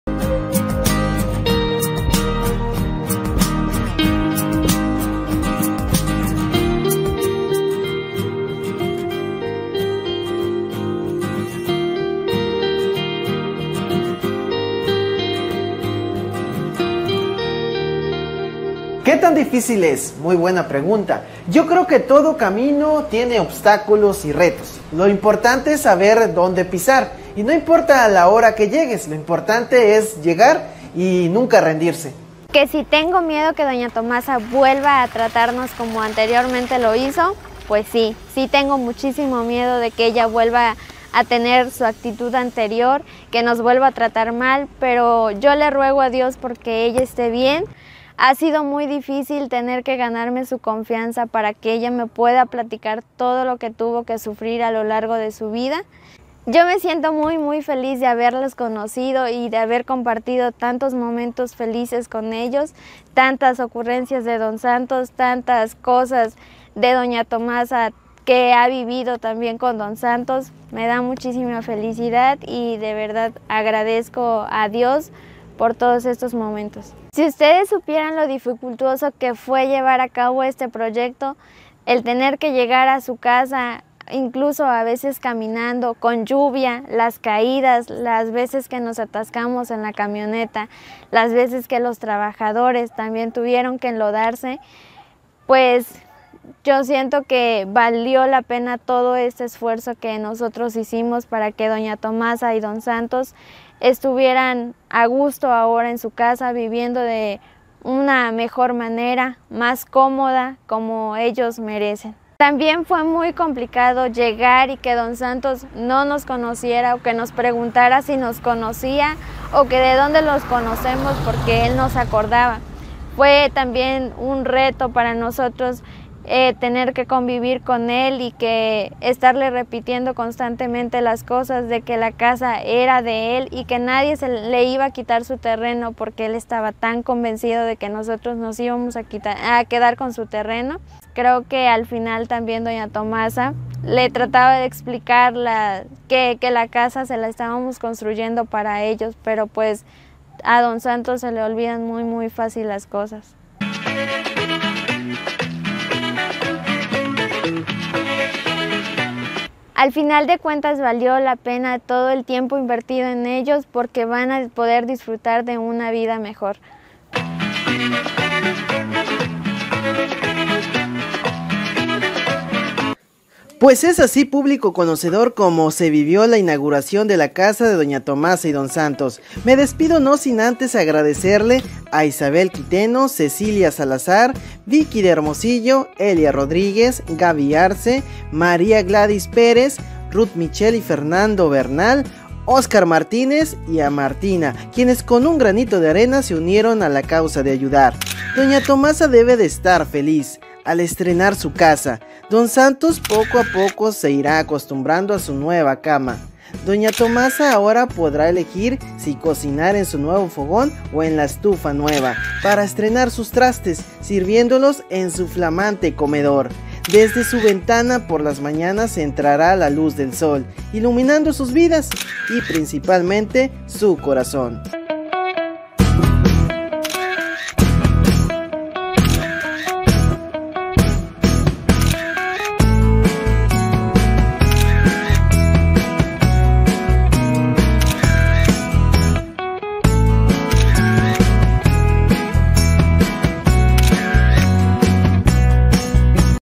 LA GENEROSIDAD DE EL CHARRO DE ORO FERNANDO BERNAL
CON SU APOYO MUCHOS ABUELITOS
 TIENEN CASA EN TANTOTUYUCA TORAL.